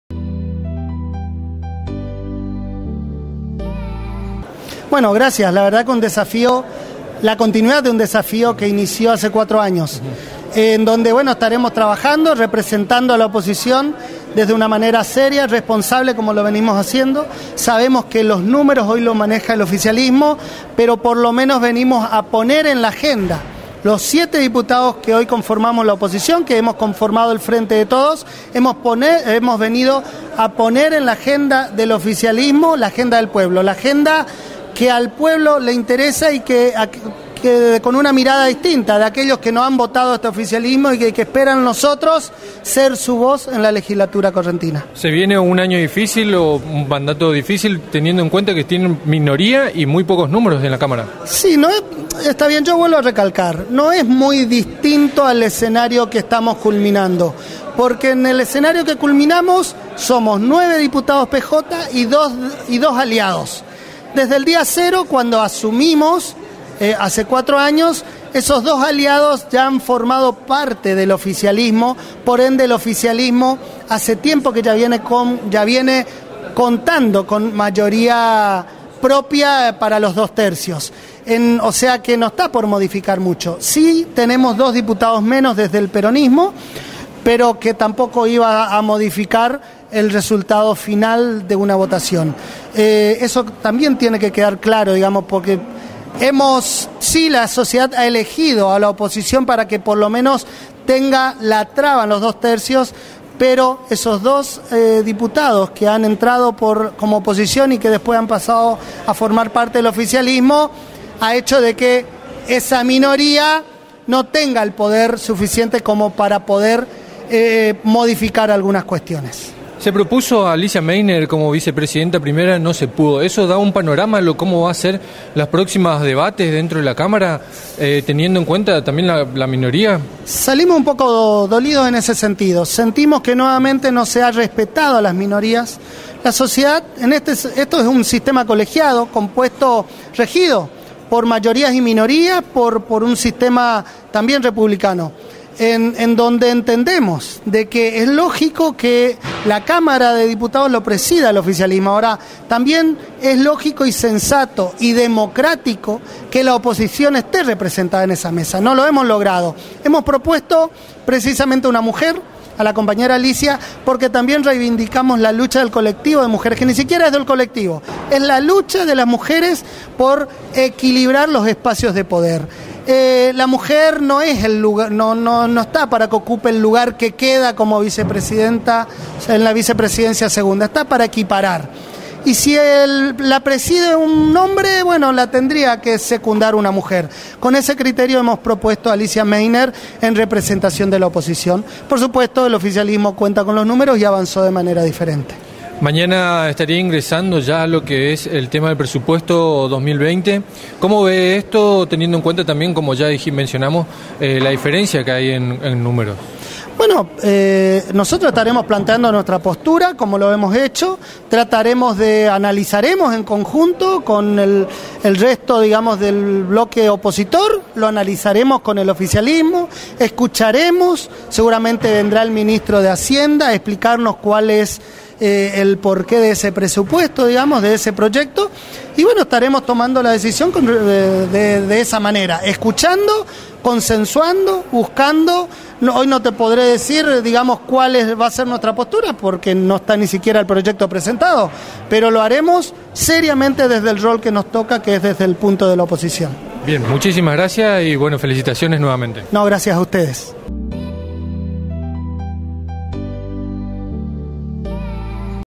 ACEVEDO CESAR
DIPUTADO BLOQUE PJ ACEVEDO CESAR
EL LIBERTADOR TV

Ju­ra­ron Cé­sar Ace­ve­do (PJ); Juan Car­los Álva­rez (UCR); Mi­guel Ma­teo Arias (PJ); Nor­ber­to Ast (UCR); No­e­lia Lu­cre­cia Baz­zi No­e­lia Lu­cre­cia; Pe­dro Ge­rar­do Cas­sa­ni (E­LI); Eduar­do Har­doy (PL); Lo­re­na La­za­roff (UCR); Héc­tor Ló­pez (UCR); Ali­cia Meix­ner (PJ); Ma­riel Fa­bia­na Me­za (UCR); Mar­cos Je­sús Ota­ño (PJ); Ma­ría del Car­men Pé­rez Duar­te (E­LI); Fran­cis­co An­gel Po­des­tá (PC) y Ja­vier Lau­ta­ro Sá­ez (PP).

EL LIBERTADOR TV